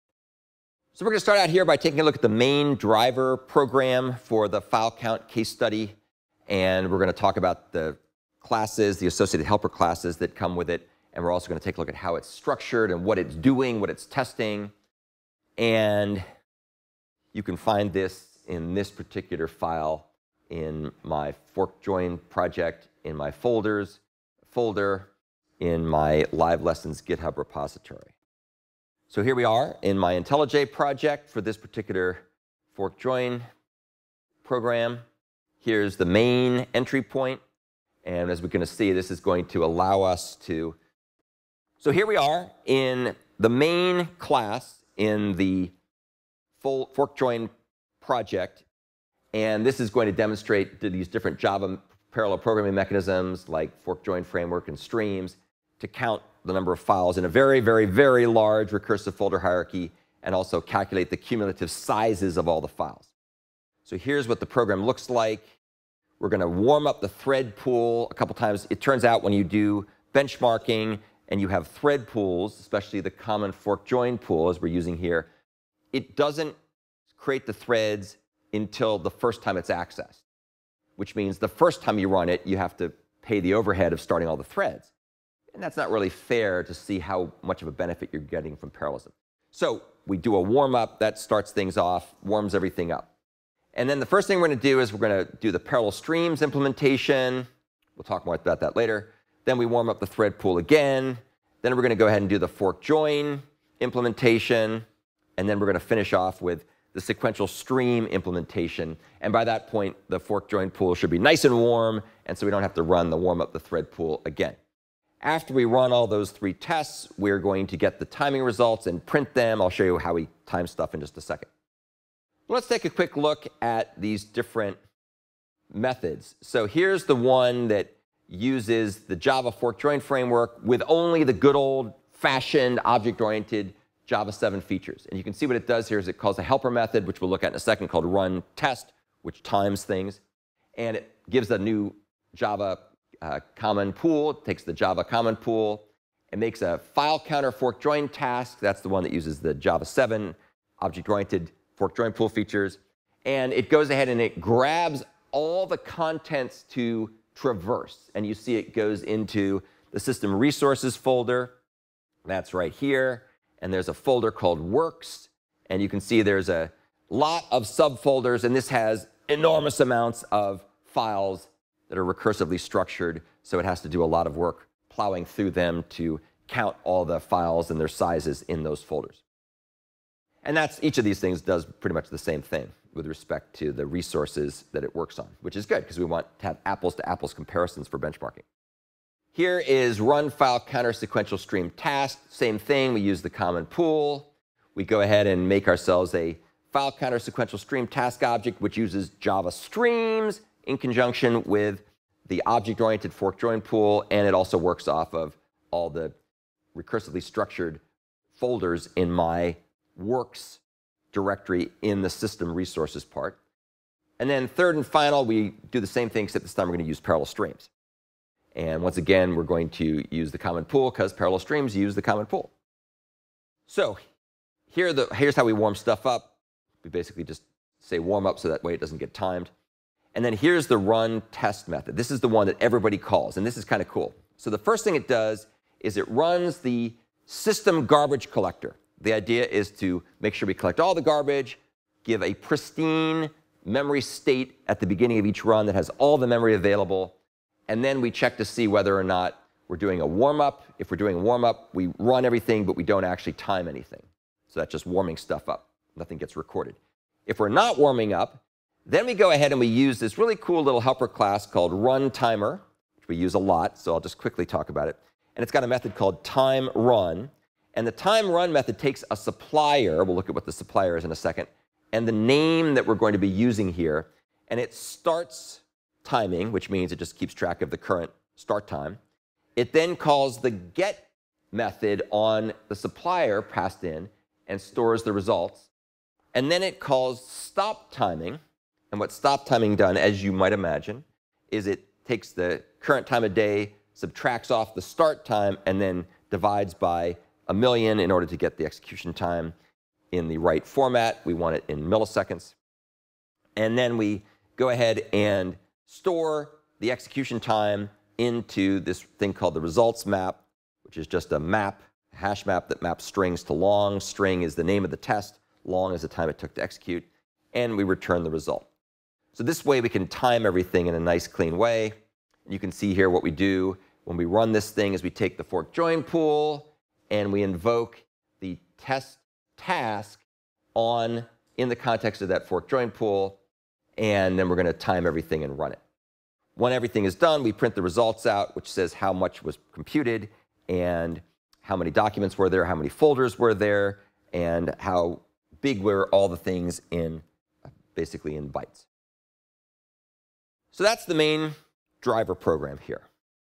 The FileCount Case Study: Main Driver Program
This video discusses the FileCount Case Study, which is aJava program designed to count files and calculate their sizes within a recursive folder hierarchy using different parallel programming mechanisms like the Fork-Join framework and parallel streams. The program starts with a "warm-up" to initialize the thread pool, eliminating any first-run overhead. Three tests are conducted: one using the fork-join framework with Java 7 object-oriented features, a second the fork-join framework in conjunction with Java sequential streams, and the third using Java parallel streams, all targeting the same system resource folder to ensure apples-to-apples benchmark comparisons. A helper class called RunTimer is used to collect precise timing metrics, which are then stored in a Java HashMap for analysis. After the three tests are run the results are printed, showing the number of documents, folders, and their sizes.

All the code shown in this video is available in open-source form